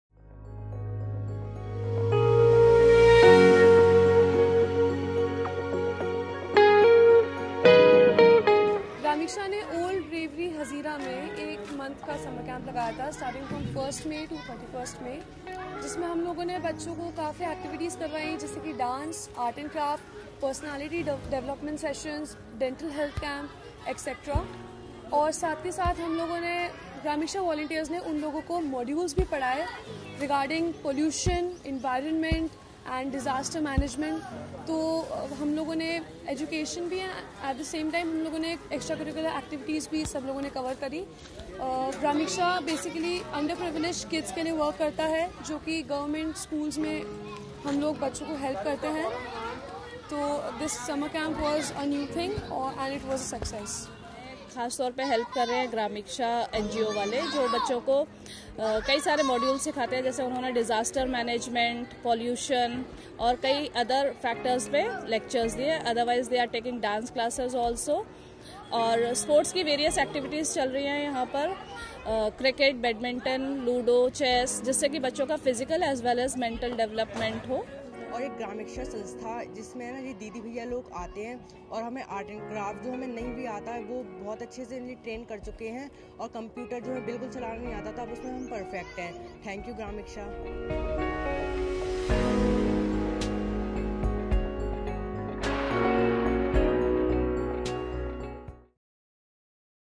Summer Camp Gwalior 2017
Here is a sneak peak to our one-month summer camp successfully organised by gramiksha, in collaboration with the government, that started from 1st of May till 31st of May, 2017.
We focused on passing education regarding general topics such as disaster management, pollution and environment, etc. At the same time, there were extracurricular activities involved too, where the gramiksha volunteers taught skills like dance, personality development and more. We also organised a dental health camp in the middle of the summer camp to give out the best of our efforts in as much time available to us.
Special thanks to Mr SB Ojha-APC(Ass. District Program Officer), Education Department, Gwalior.